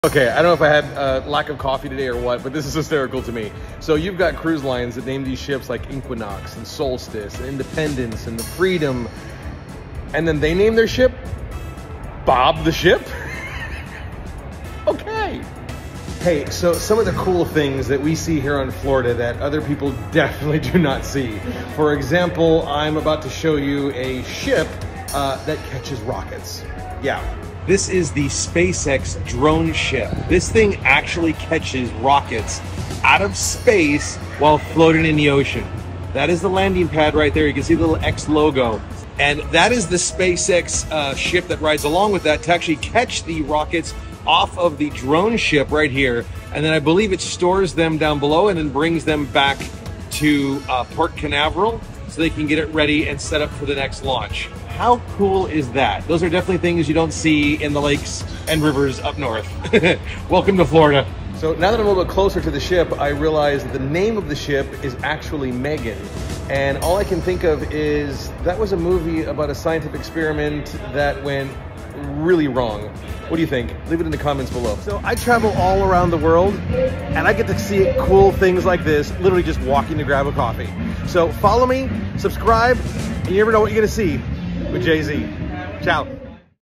Let me get this straight, multibillion-dollar company named it, SHIP Bob! Ha ha
Come on SpaceX I expected you to do better than naming your ship Bob lol so I’m in Port Canaveral and I unexpectedly notice SpaceX ship sitting in the port pretty cool experience, but after about 20 minutes of looking at the ship, I realized what the name of it was and I couldn’t believe the two names that were chosen I think they could’ve put a little bit more effort into them don’t you think?